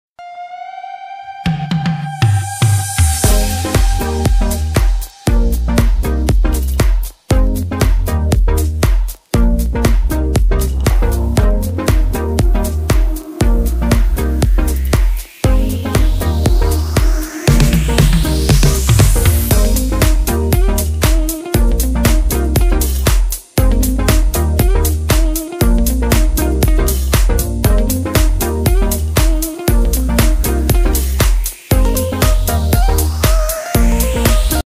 PR Calblanque: Descubriendo búho real
En 2020 el Búhoreal volvió a nidificar en la pared del CabezodelaFuente, 10 años después de la última cita de su reproducción en ese lugar. Compartimos el emocionante momento en que voluntarios de la
AsociacionUlula encuentran plumones y egagrópilas de los polluelos.